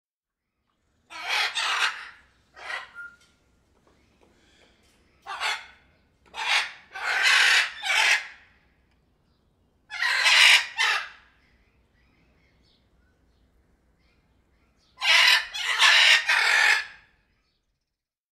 Bird, Parrot - Small Group of Blue and Yellow Macaws: Vocalization, Birds
Bird, Parrot - Small Group of Blue and Yellow Macaws: Vocalization, Birds · Sound Effects Library

Birds: Sound Effects